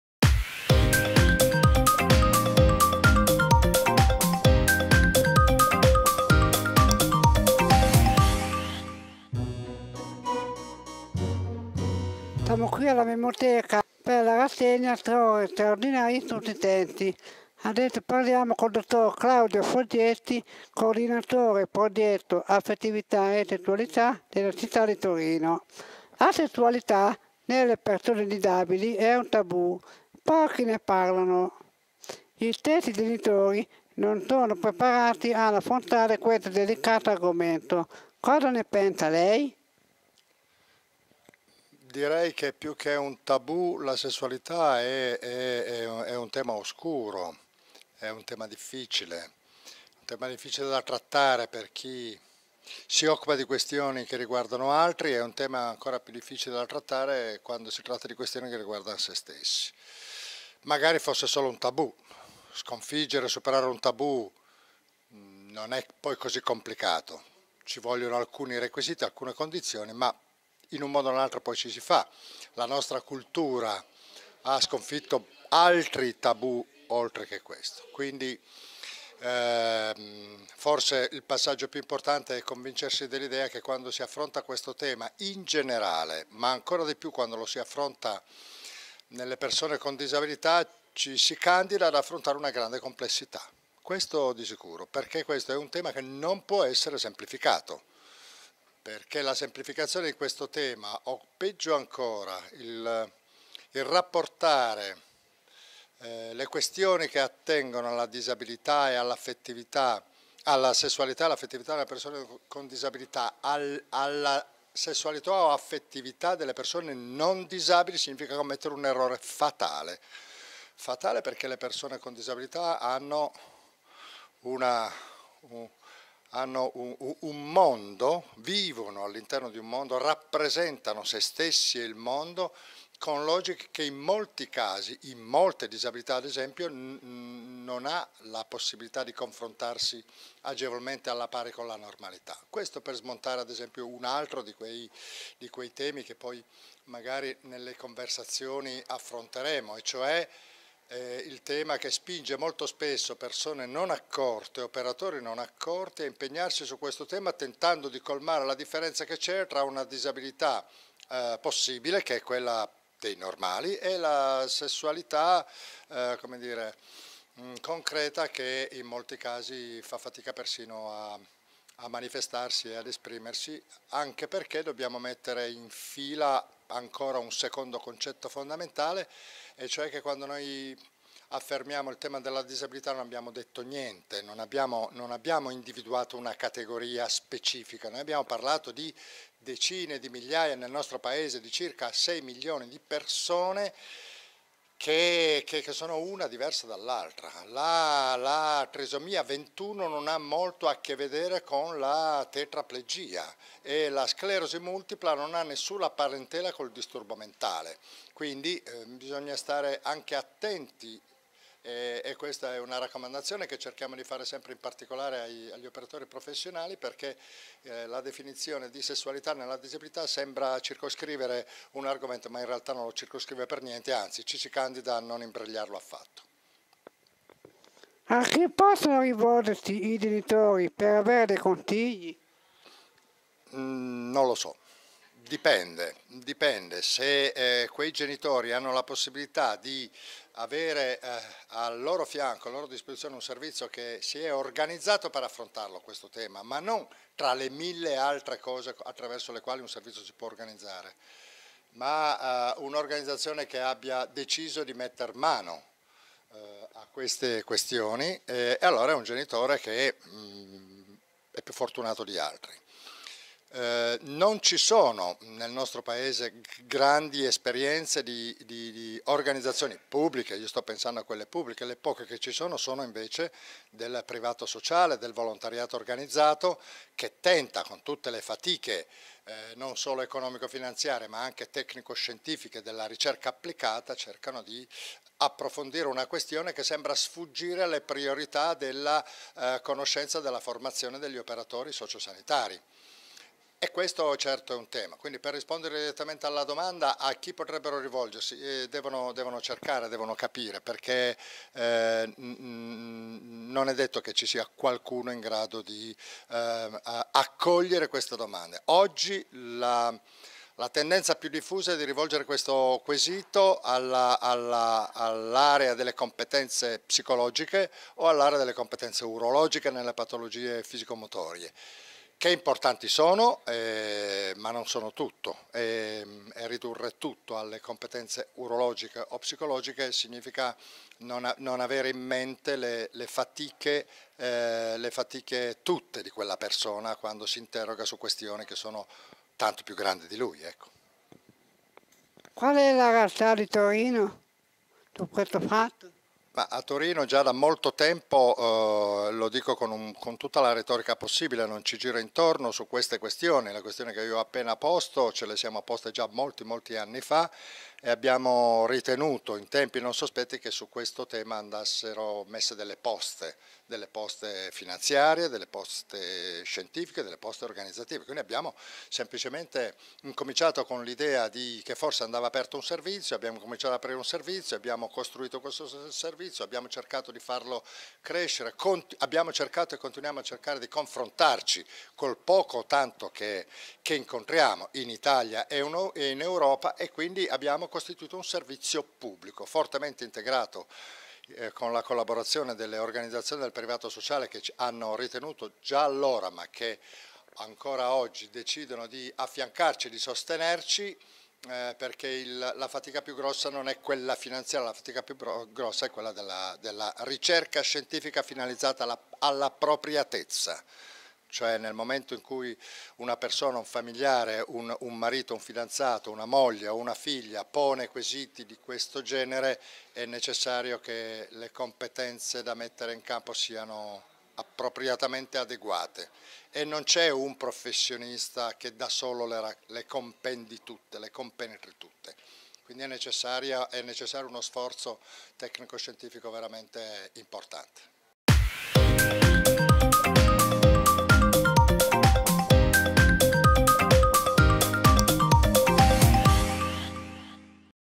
Sessualità e disabilità- Claudio Foggetti ai nostri microfoni
Il dott. Claudio foggetti Coordinatore del Progetto "Affettività e Sessualità" di Torino spiega quanto questo sia un tema oscuro e difficile da affrontare. Non possiamo rapportare la sessualità di persone abili a quelle disabili.  In Italia ci sono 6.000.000 di persone con disablità e ogni adisabilità è differente dall'altra e quindi ha bisogni e attenzioni diverse anche dal punto di vista dell'affettività.